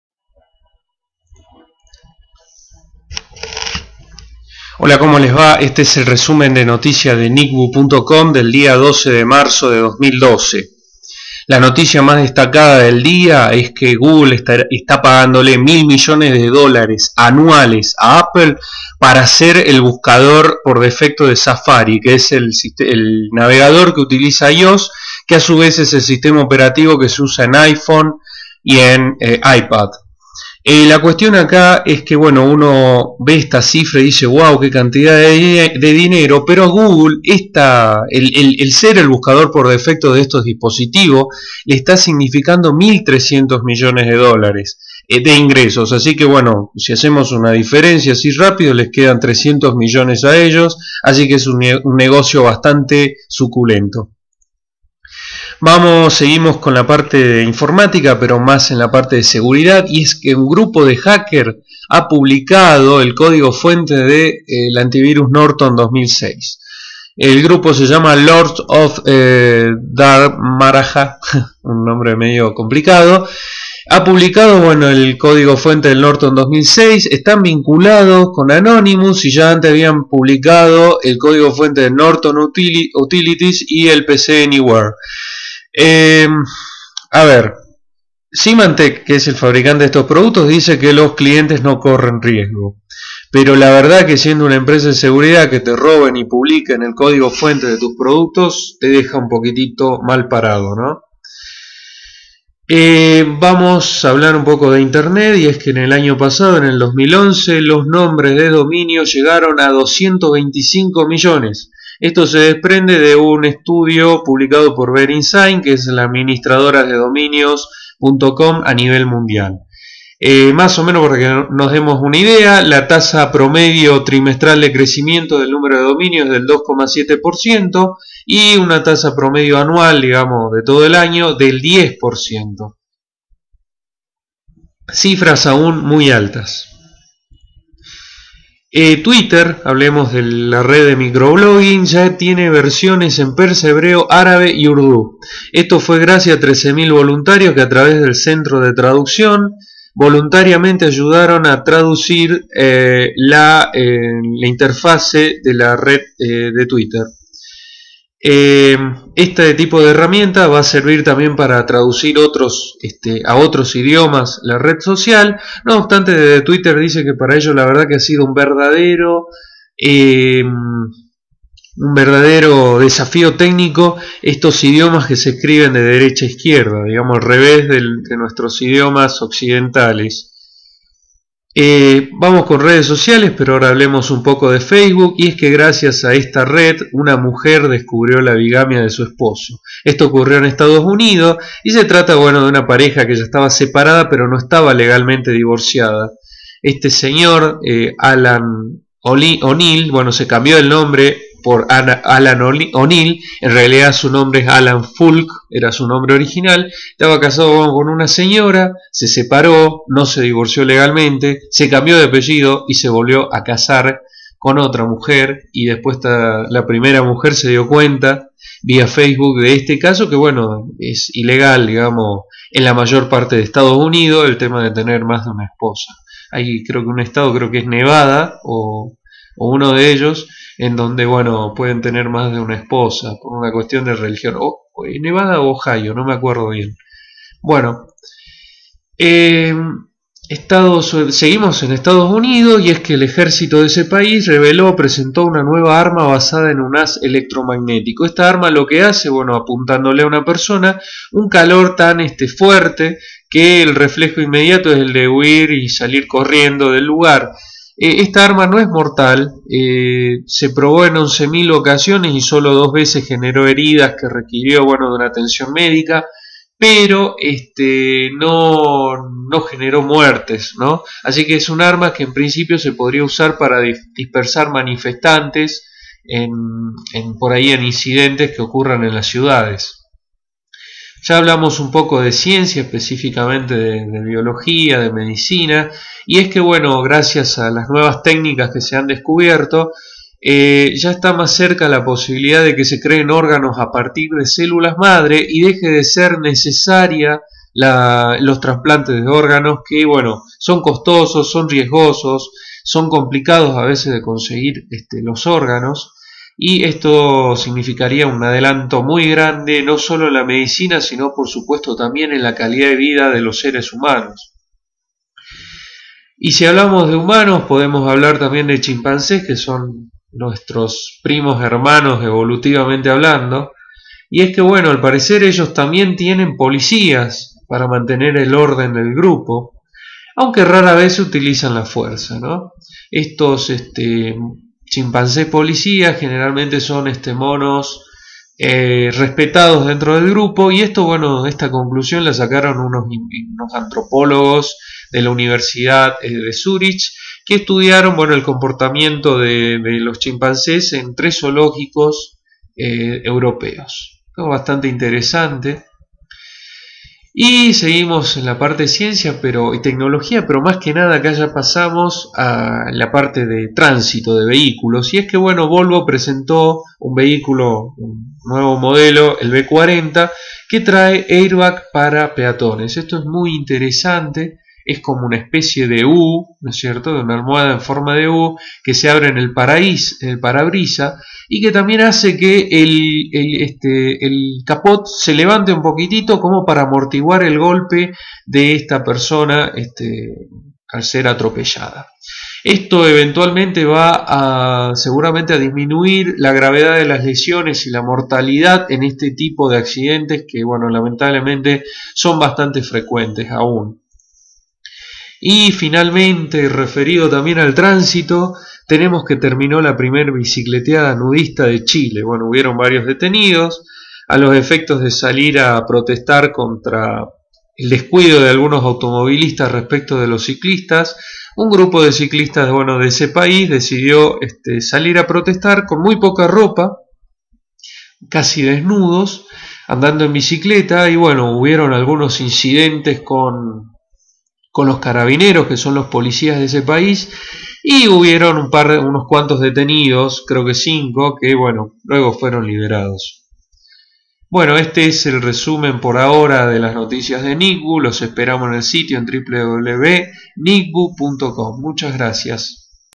Destacada: Google le paga a Apple 1.000 millones de dólares anuales para ser el navegador de Safari
Este es el resumen de noticias del día 12 de marzo de 2012 de Nicboo.com, a continuación tienen los enlaces a los posts completos para poder profundizar en los temas:

No falta mucho para que se puedan crear órganos con células madre

Un grupo de hackers publicaron el código fuente del antivirus Norton 2006

El Ejército de Estados Unidos reveló una nueva arma basada en haz electromagnético

Google paga U$S 1000 millones anuales por ser el buscador de Safari de iOS